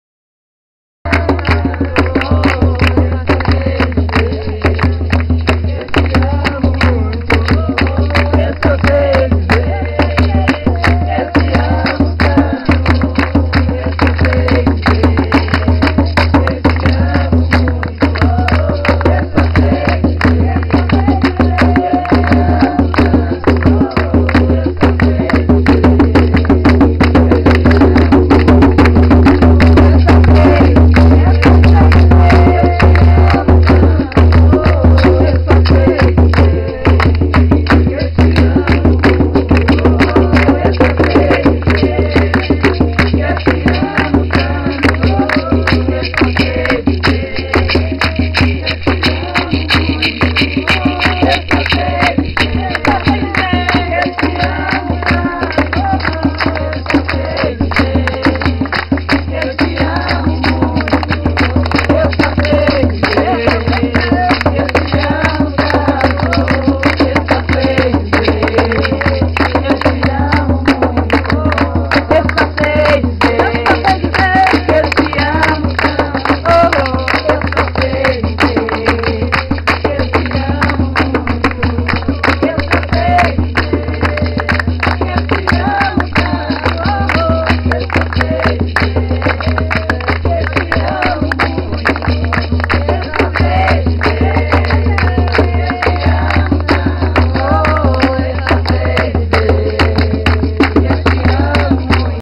Ritual Xamânico - Homenagem a São Francisco
Dia 04 de outubro de 2009, dia de São Francisco - Ritual na Comunidade Educadora Terra Mirim, em Simões Filho - Bahia, com a Xamã Alba Maria.